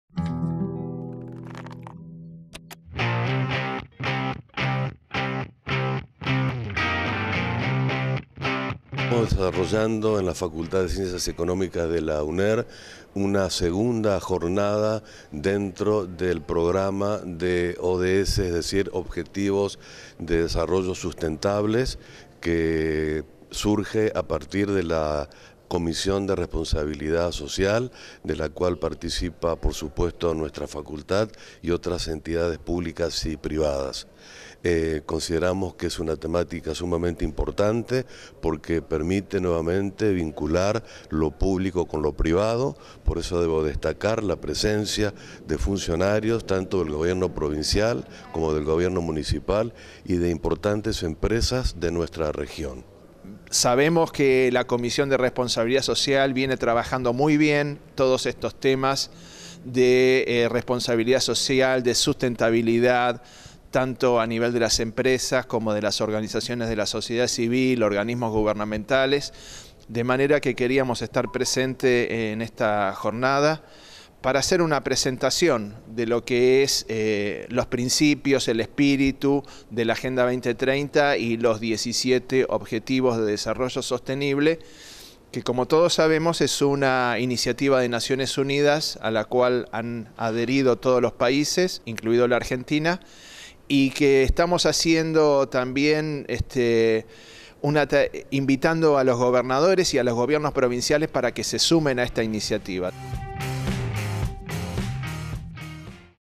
Objetivos de Desarrollo Sostenible
Se desarrolló la jornada “Los Objetivos de Desarrollo Sostenible – Desafíos para la implementación local”,  en el Aula Magna en la Facultad de Ciencias Económicas.

Fue organizada por la Comisión de Responsabilidad Social del Consejo de Políticas Sociales de Entre Ríos, que integra nuestra Universidad.

La actividad estavo conducida por la Gabriela Agosto (Secretaria Ejecutiva del Consejo Nacional de Coordinación de Políticas Sociales – Presidencia de la Nación), responsable de los ODS – Agenda 2030 de ONU en Argentina.